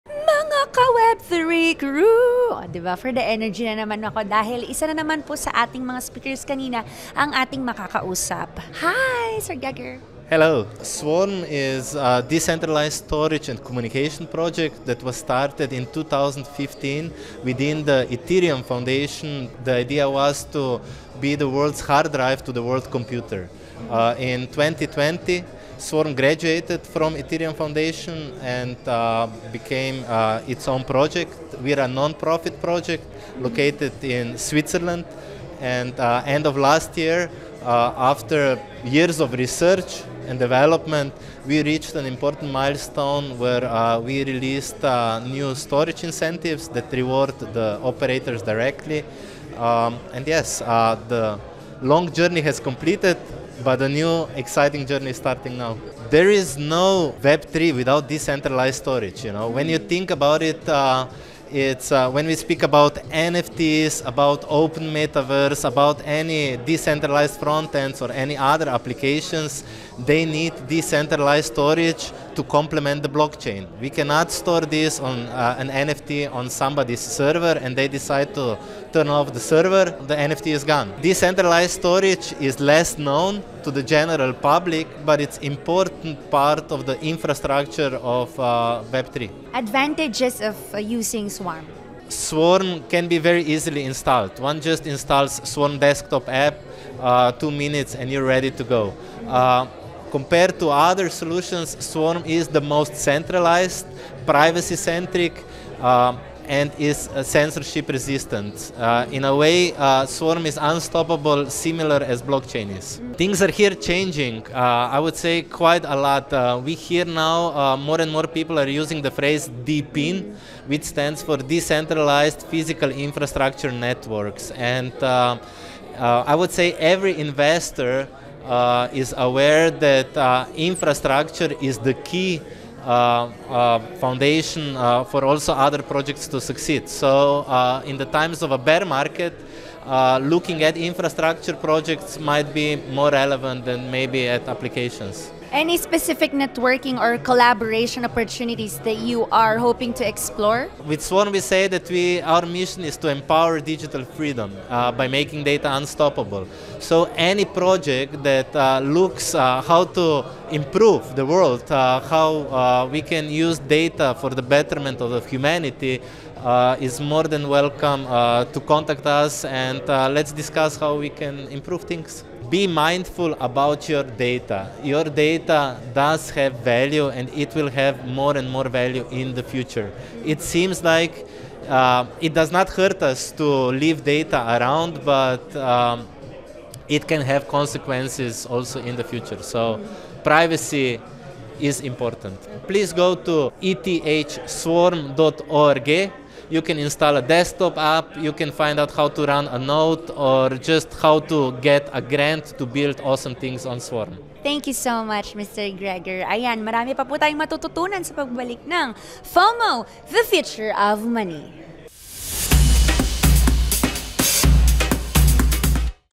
Gregor Zaucer at XT.com | FOMO: In Focus
Check out Krista 2 Moons Interview with Gregor Zaucer of Swarm at XT.com Meet-up.

Connect with PinoyWeb3TV on your favorite social media platforms!